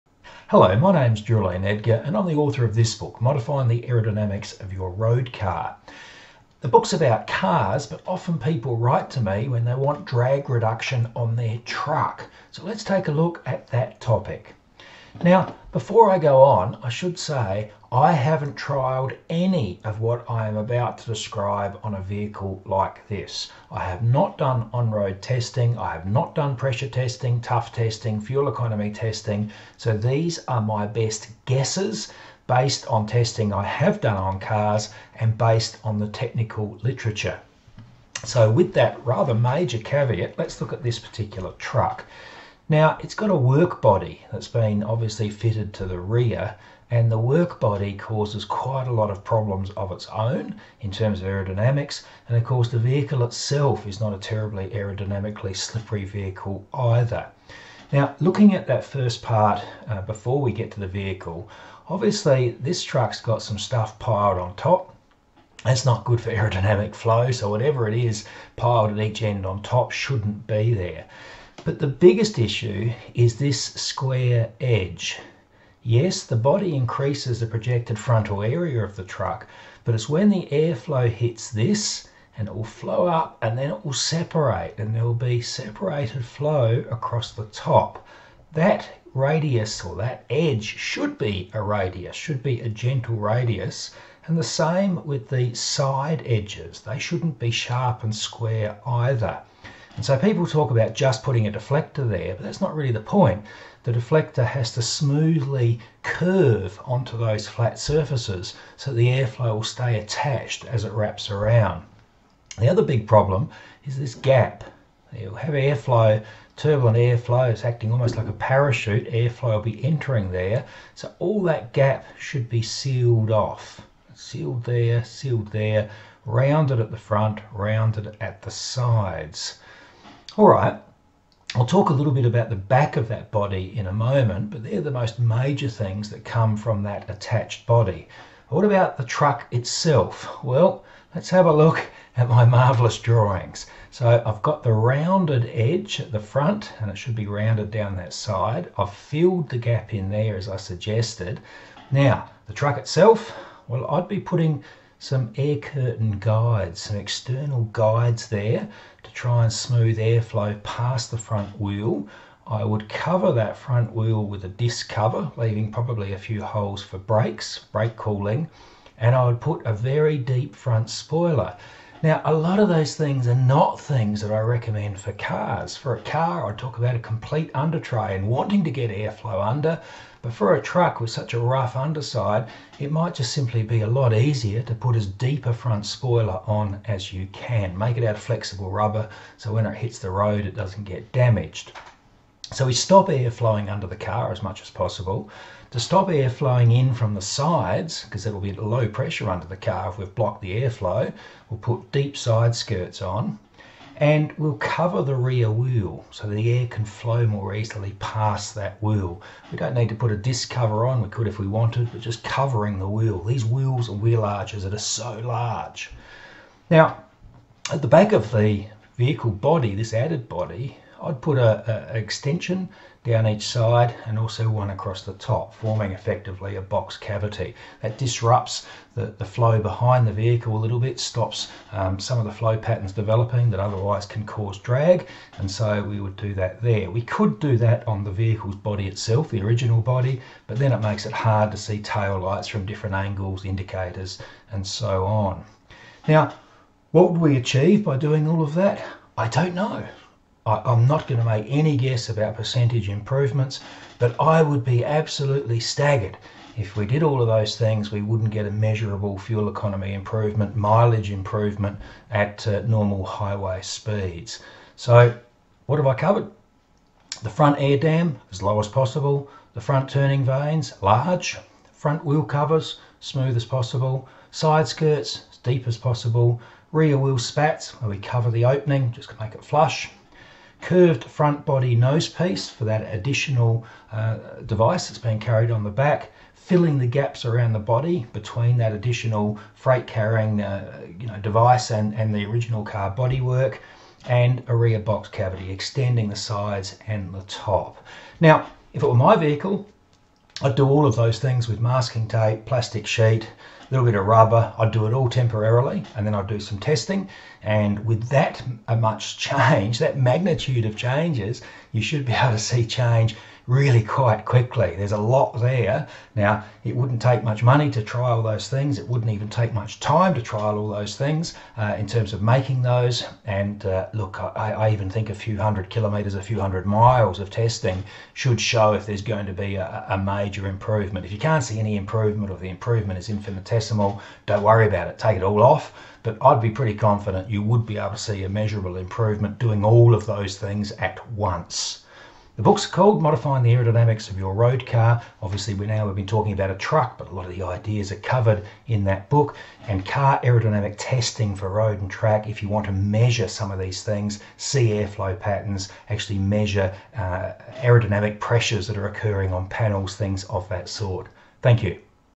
Drag reductions on a truck
Cheap and easy techniques to potentially decrease aerodynamic drag.  Note that my most recent aero book (replacing the one shown in this video) can be found or Amazon in your country.